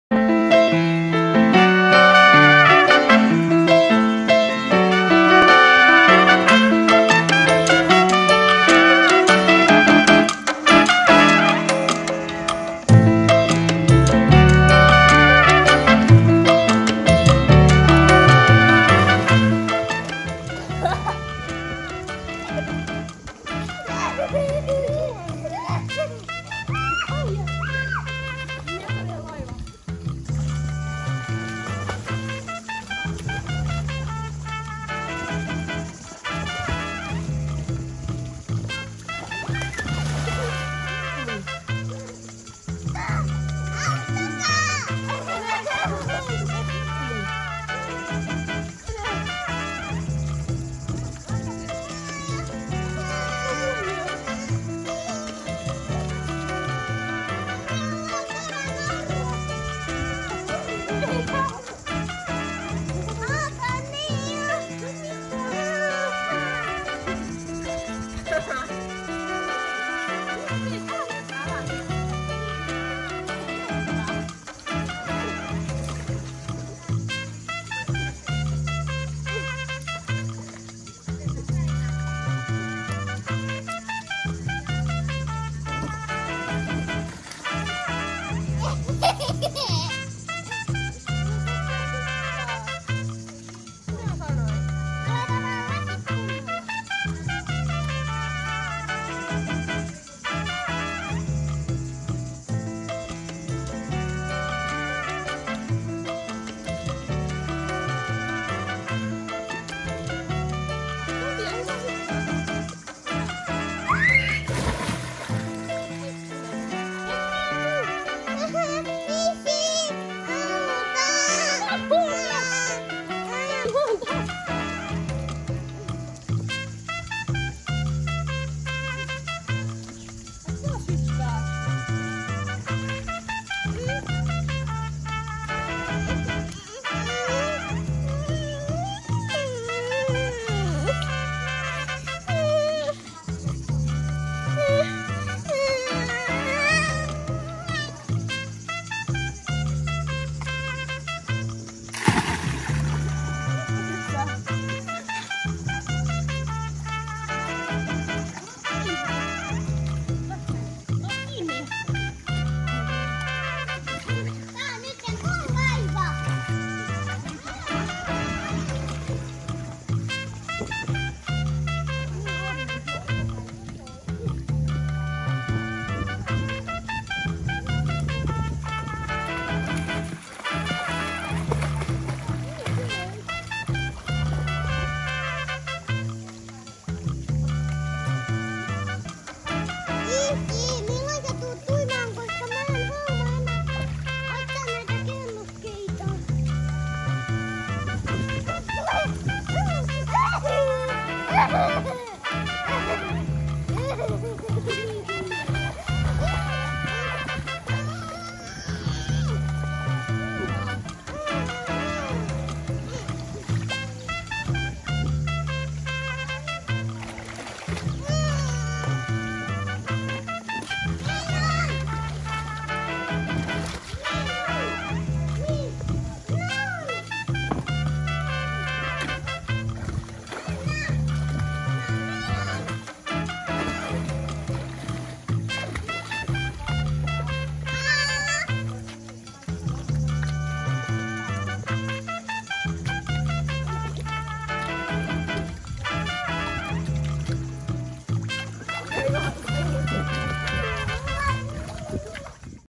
Nea & Sofia Holiday. Albania Ksamil
Lomailua Albanian pienessä ksamil kylässä. Upea pieni lomaparatiisi aivan eteläalbaniassa. Lämpöä 36 astetta joten pieni pulikointi on aina paikallaan.

Best holiday on Ksamil beach at Albania. Ksamil, Sarandë